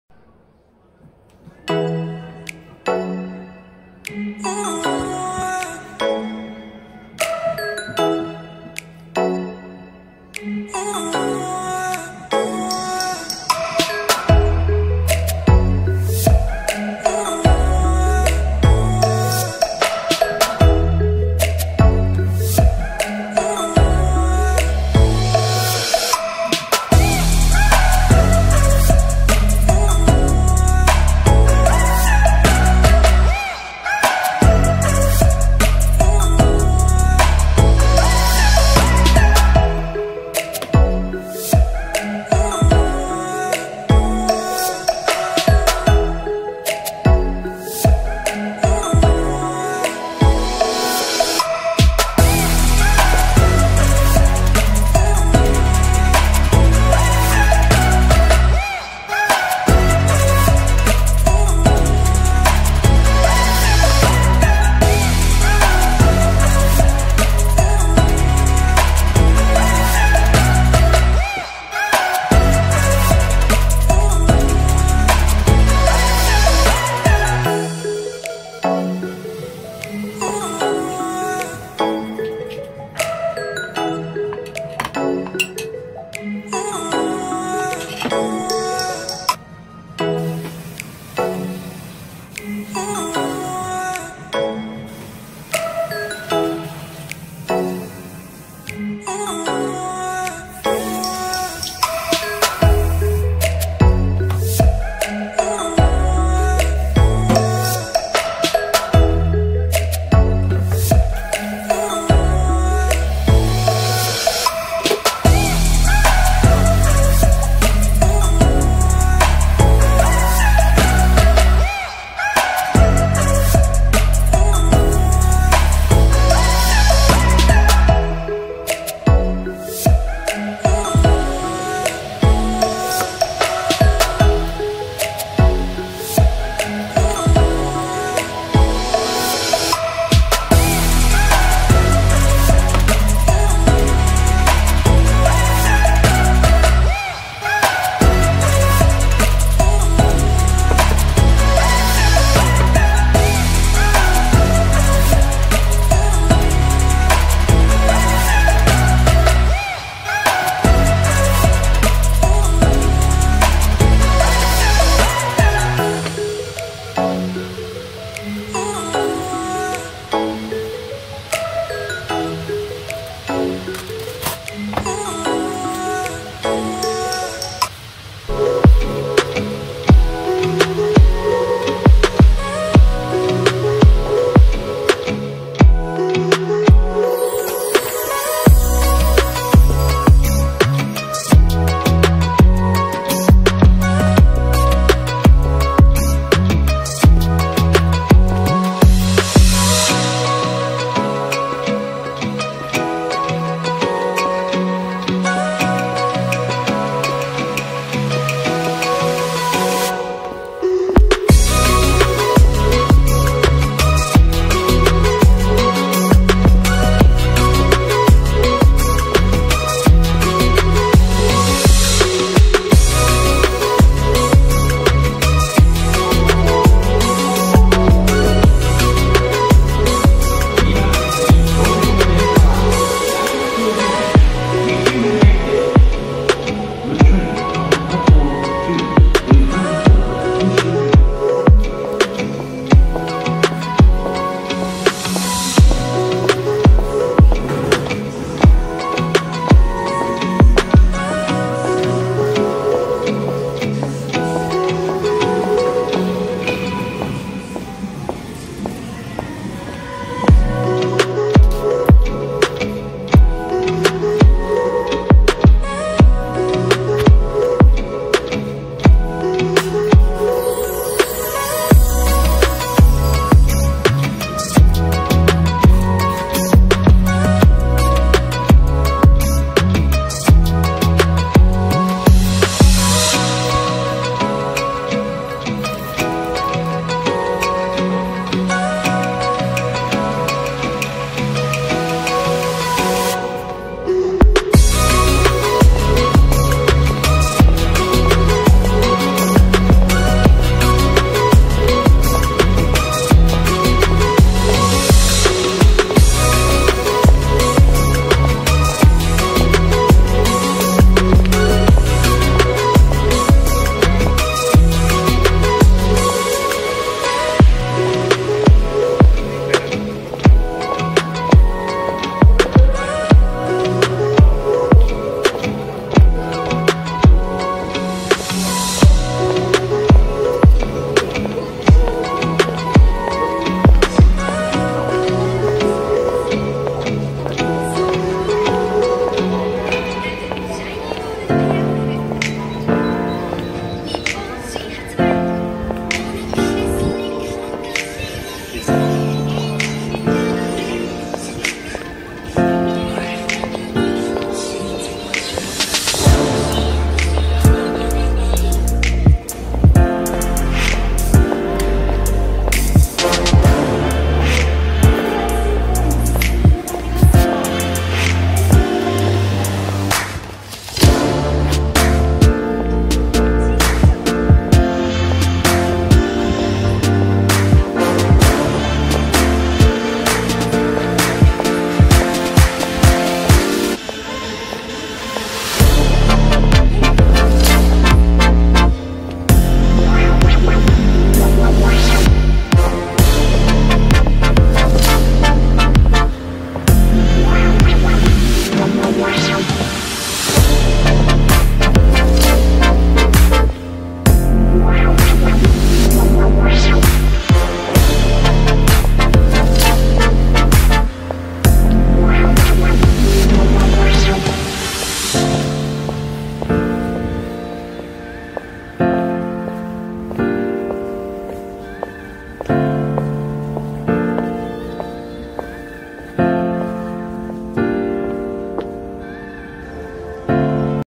Walking around Dotonbori | Rest day vlog | Teacher in Osaka
This is Vlog #2!!!
It's shorter than my first video, and it took me some time to shoot and upload a second video.
- Angelie